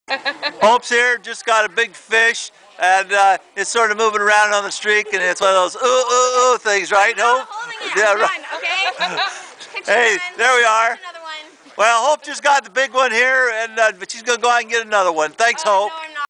Free Fishing Day in Oregon
Catch of the day at Cleawox Lake Honeyman State Park.
Siuslaw School 5th and 6th Grade students.